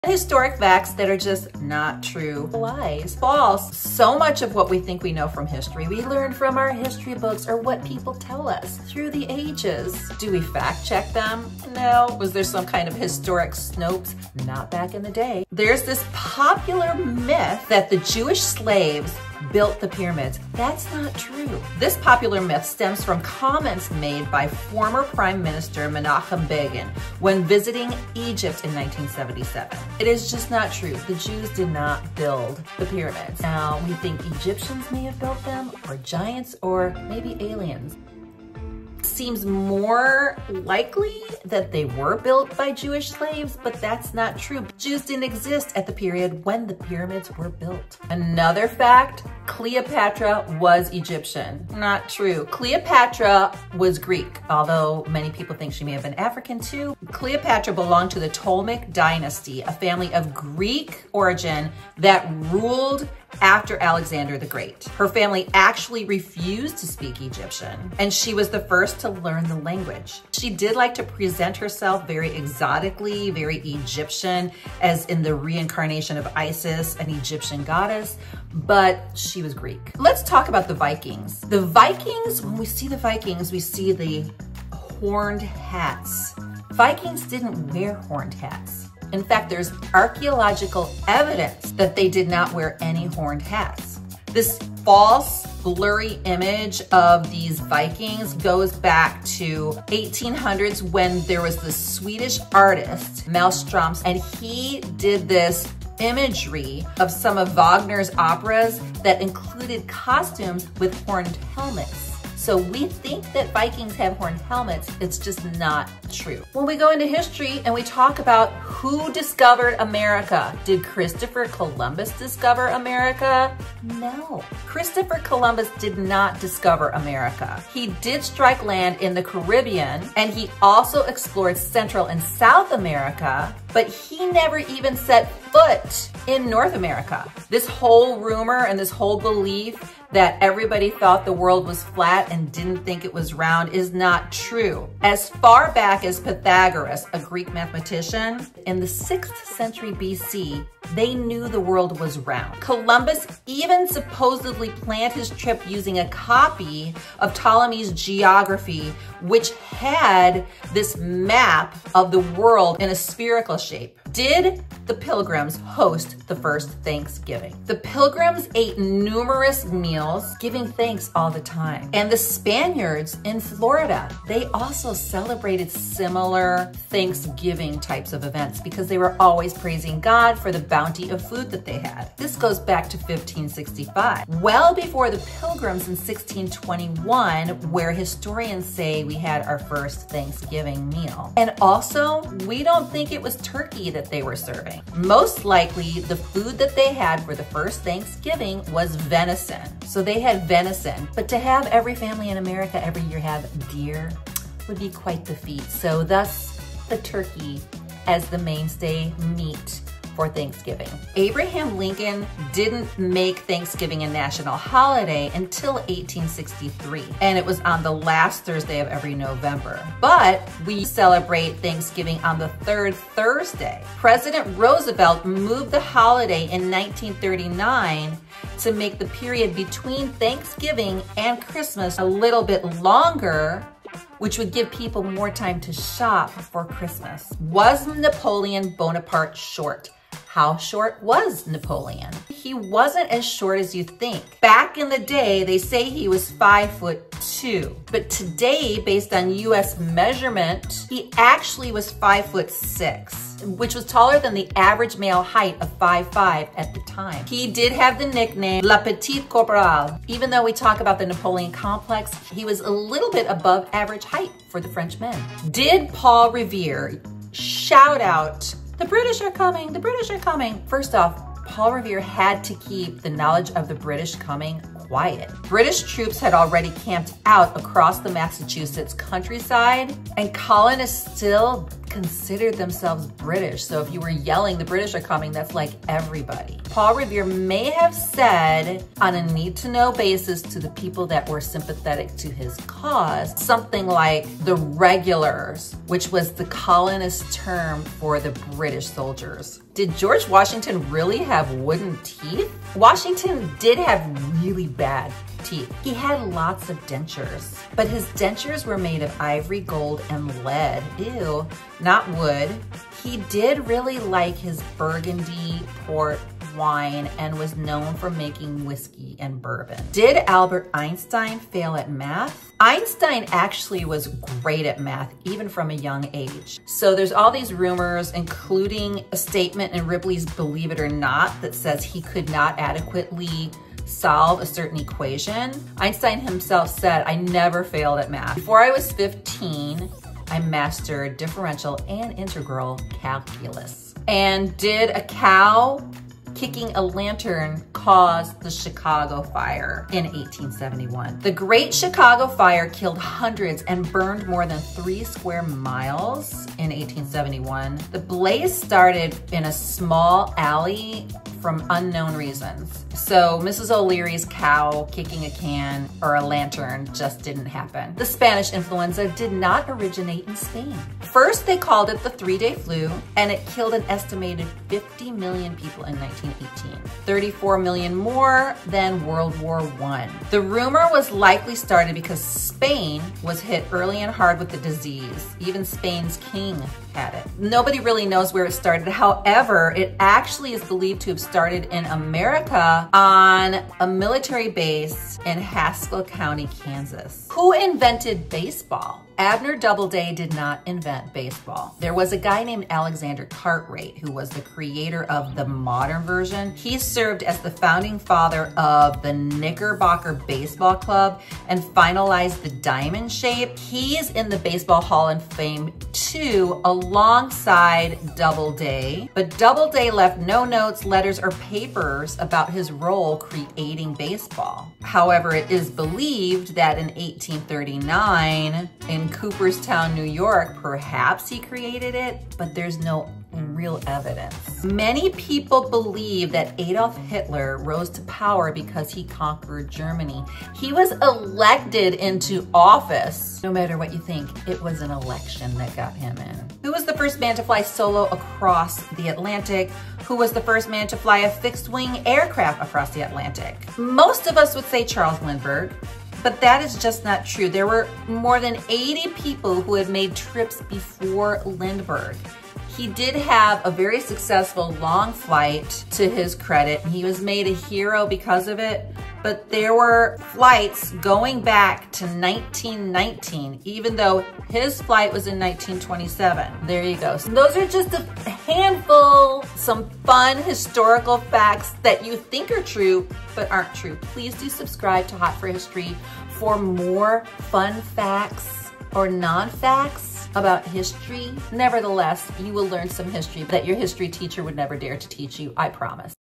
Top 15 Shocking Lies About History!
Top 15 Historical FACTS That Aren't True.

Were the Pilgrims Really the first to have thanksgiving?
Did George washington really have wooden teeth?
What nationality was cleopatra?
Did Jews build the pyramids?
Did Columbus Discover America and more...

Our Books:
Henry's Children and Other Stories
Spanish Flu: 19 Days in 1919

00:00 Intro
15:12 Did Jews build the pyramids?
48:04 Was Cleopatra Egyptian?
1:19 Did Vikings wear helmets with horns?
1:56 Did Christopher Columbus discover America?
2:17 Did People think the earth was flat when Columbus was exploring
2:44 Were the Pilgrims the first to have Thanksgiving in 1621
3:57 How short was Napoleon?
4:24 Did Paul Revere really shout "the british are comiing"?
4:59 Did George Washington have wooden teeth?
5:19 Was Albert Einstein bad at math?
5:43 Did Mrs. O'Leary's COW cause the Chicago fire in 1871?
6:07 Did the Spanish Flu start in Spain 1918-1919
6:37 Who invented baseball?
7:14 How did Adolf Hitler Rise to Power?
7:26 Was Charles Lindbergh the first pilot to cross the Atlantic?